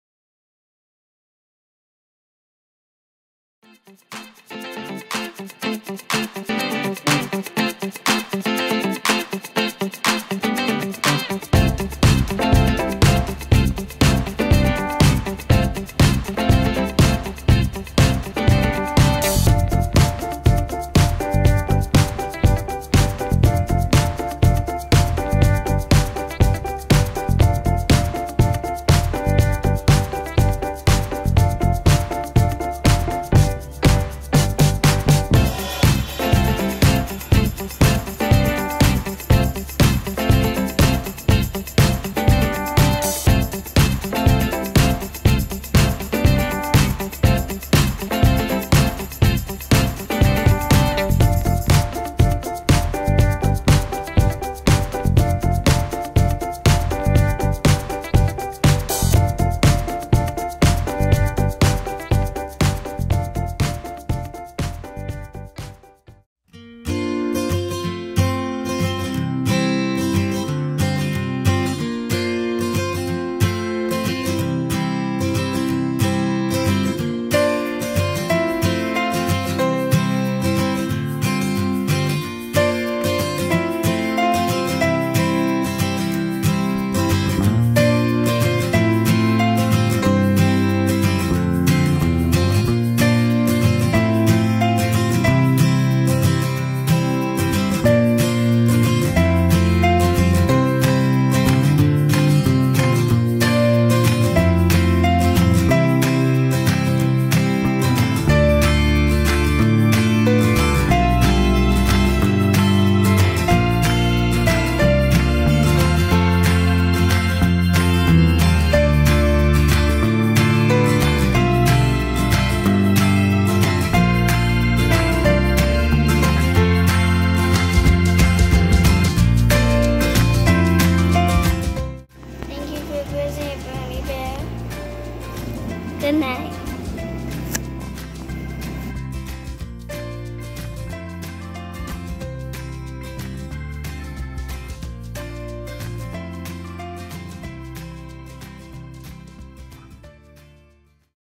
Brownie Bear and Gavin
Brownie Bear is Gavin's Kidergarten Class room Bear.  Brownie Bear spent the weekend with Gavin and the family.  Here's a little video of his adventures with us.